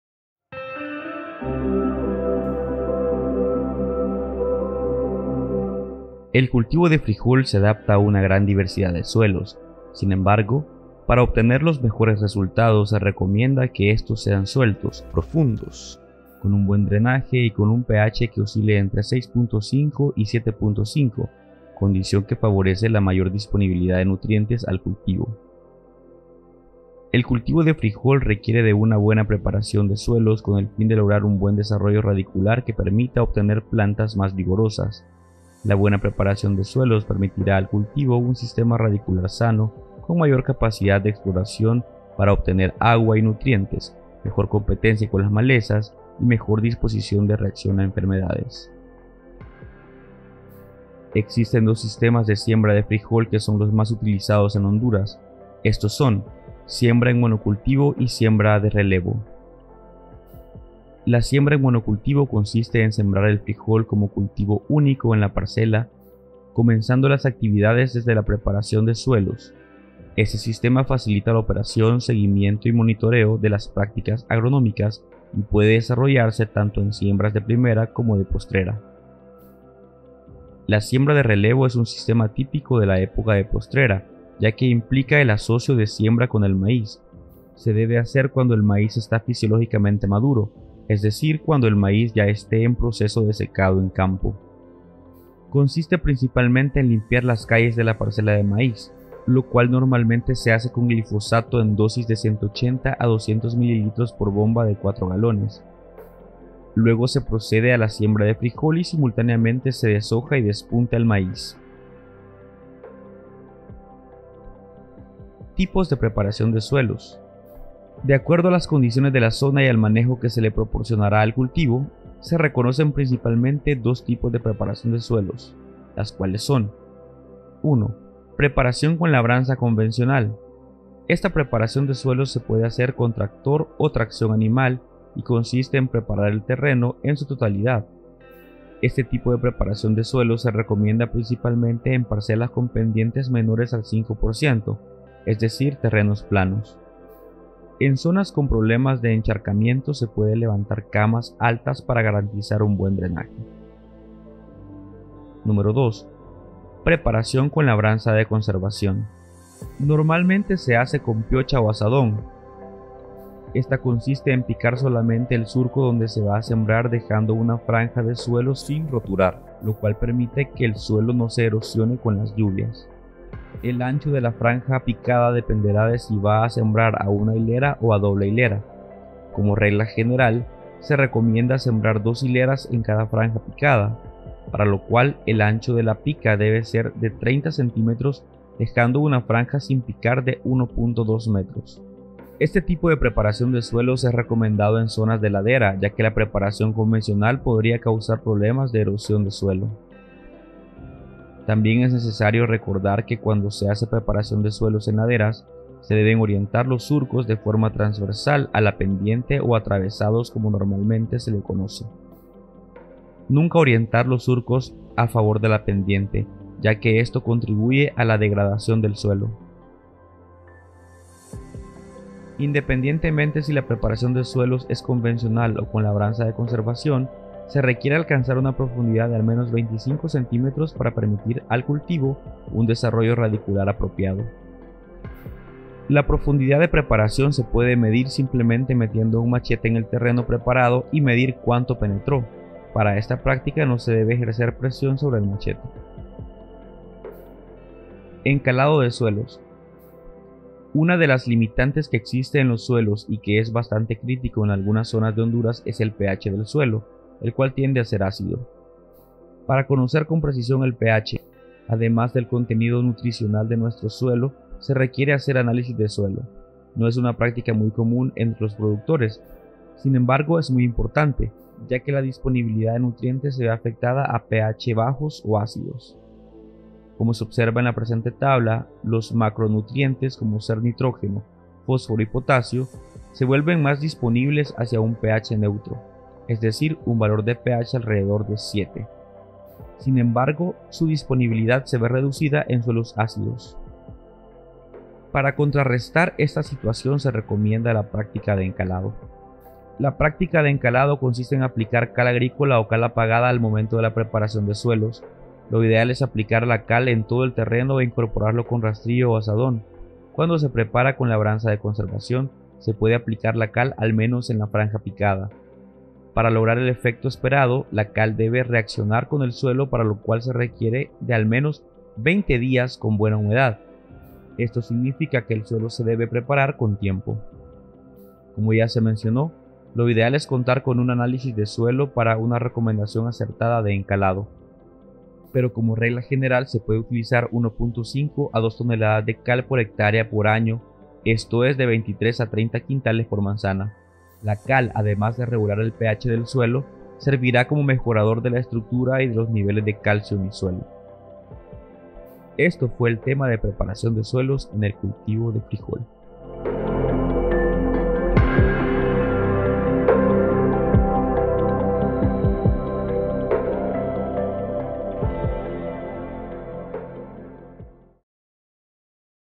Tutorial 1 Frijol: PREPARACIÓN DE SUELOS
Videotutorial para que el productor de frijol pueda aprender técnicas adecuadas para aprovechar mejor su cosecha de frijol, en este tutorial aprenderá sobre las características de un suelo correcto y como debe prepararse para sacarle un mejor rendimiento a su cultivo.

Cada semana se estará subiendo un nuevo tutorial, y se dejará el enlace al final de cada video para el siguiente tutorial.

Por favor déjanos un comentario si te pareció útil esta información y también sobre los temas que te gustaría ver en futuros videos.

Dirigido por el ingeniero agrónomo Rubén Yanes
Realizado por Linder Diamar
Casa Productora: Cam Somnia Films